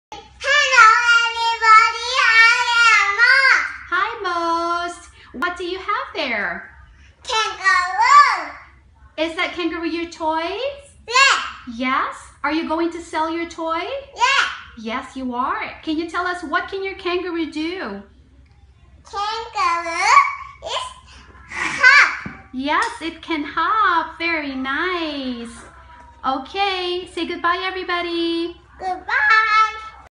Young Heroes: Old Toys for New Chances
Helen Doron English Students at Central Rama 9, Bangkok, Thailand are young heroes!! They donated and sold their toys to raise funds for students in needs.  They managed to raise 4,320 Baht and contributed it to the scholarships at Yuvabadhana Foundation.